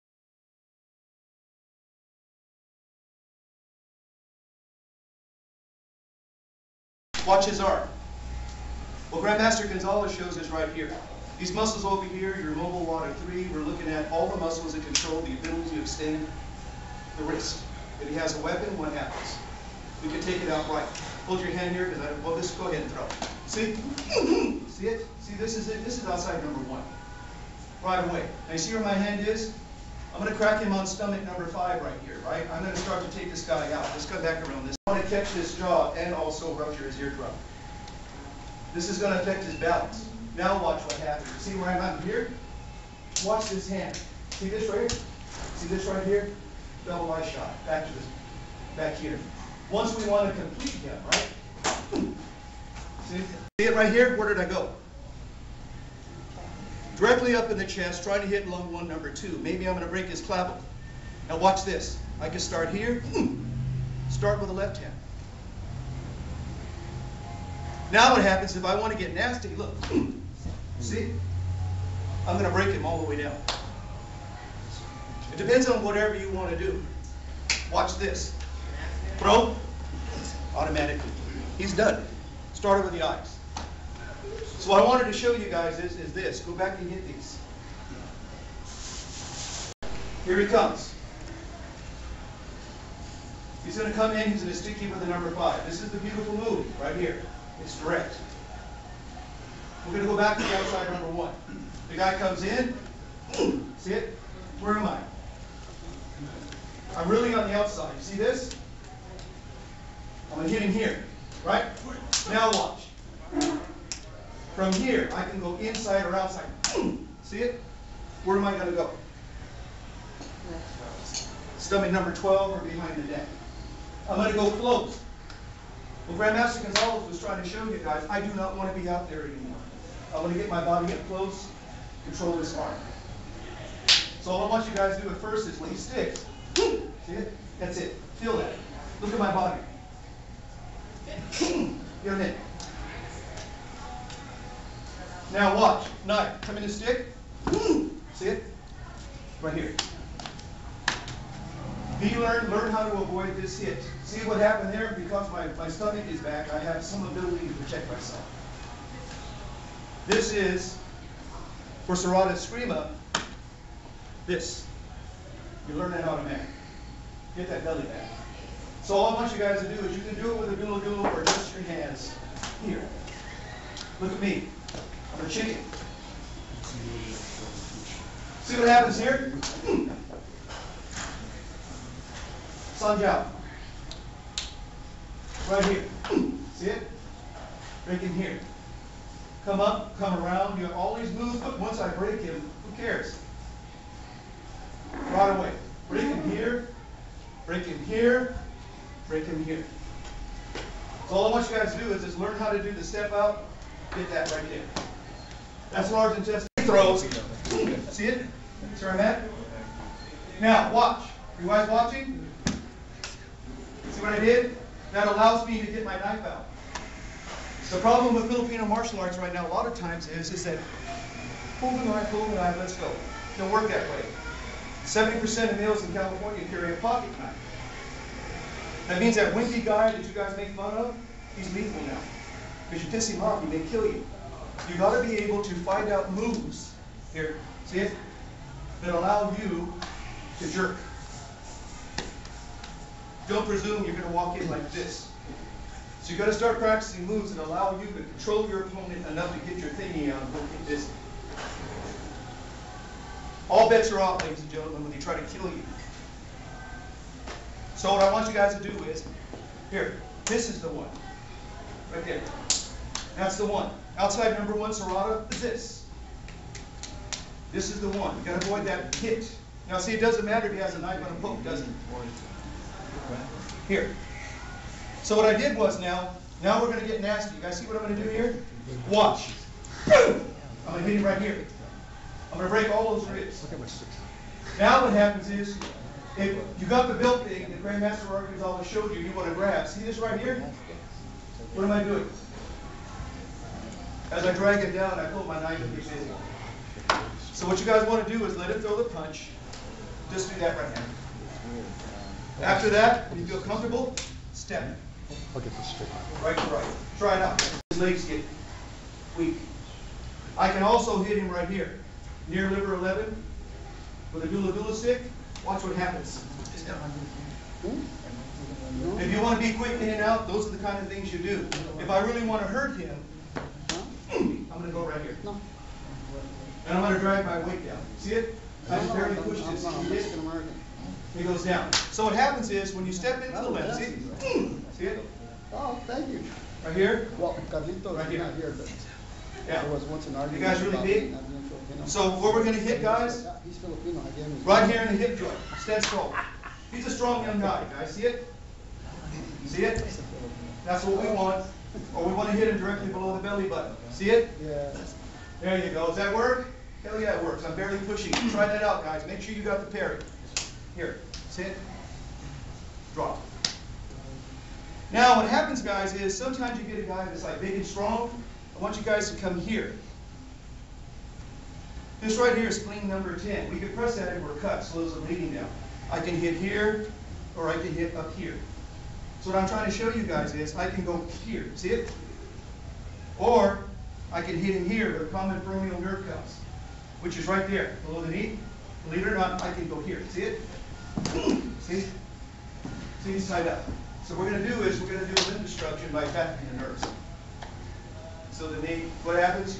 Saturno 2011 Gathering morning session
GM Ron Saturno teaching during the morning session at the Gathering in Sacramento, Ca.  Sept. 24,2011